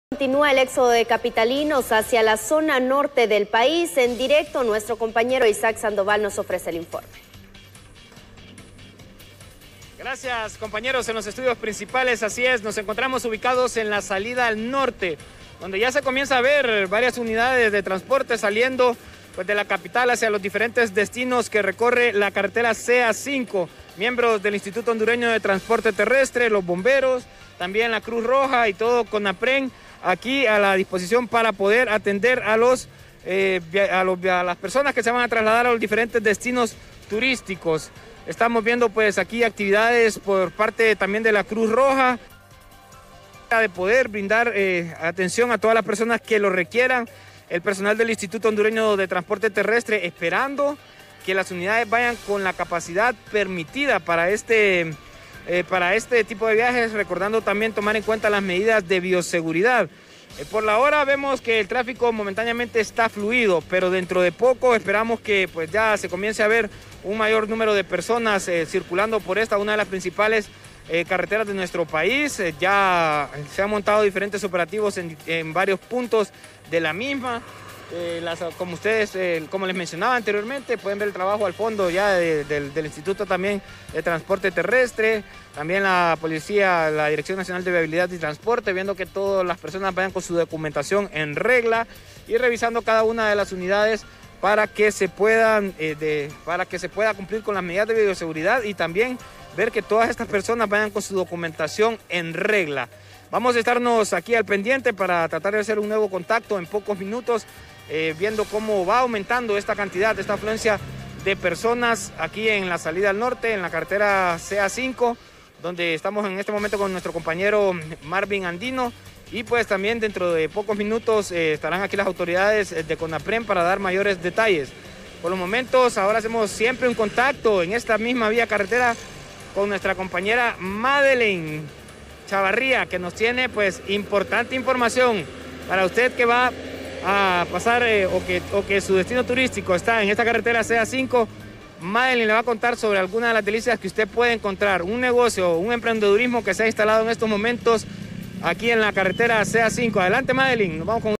Continúa éxodo de capitalinos hacia la zona norte
Hay gran afluencia de transporte público en la salida norte de la capital
Es evidente la presencia de los puesto de control del Instituto Hondureño de Transporte Terrestre y bomberos.